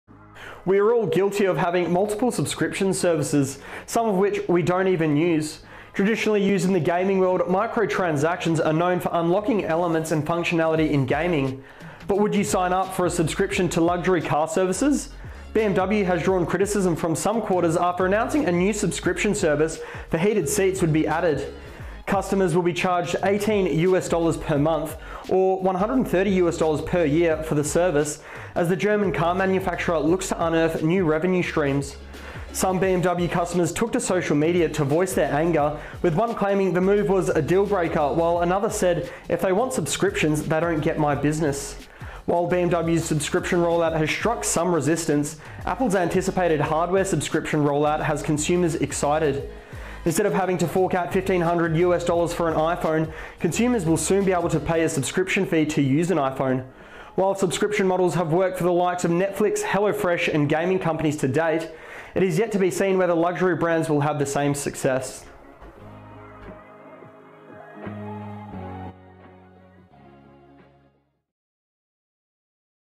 This company wants to warm your buttocks... for a fee
We are all guilty of having multiple subscription services, some of which we don’t even use but are too time-poor to cancel.

Now, the world of microtransactions is expanding out of the gaming world and into our everyday lives.

Traditionally known for their role in video games, microstransactions were used to unlock specific gaming features or specific user abilities.

Now, we have the likes of BMW joining the microtransaction bandwagon for the heated seats service.

From this month, new BMW car buyers will have the option to sign up to a monthly subscription service costing around US$18/month (AUD$26.05/month) in order to get heated seats in the front of their vehicles.

Other subscriptions BMW is offering through its digital stores in countries including the UK, Germany, NZ and South Africa include driver assist features for AUD$60.59/month, map package update AUD$136.77/month and IconicSounds Sport for AUD$171.40/month.

Some BMW enthusiasts have slammed the luxury car brand over the introduction of the heated seats subscription service, with some taking to social media saying: ‘This is a dealbreaker for me’ and ‘If they want subscriptions they don’t get my business’.

BMW is not alone, with tech giant Apple reportedly eyeing a move into the subscription service model.

Details are yet to emerge of how the subscription service will operate but it is expected to cover multiple services.

In a positive signal for the company, the move has been welcomed by Apple consumers amid increased personal living costs.

While we have seen great success come from subscription-based business models in the form of Netflix and Spotify, convincing customers of subscriptions to luxury brands may prove a little more challenging.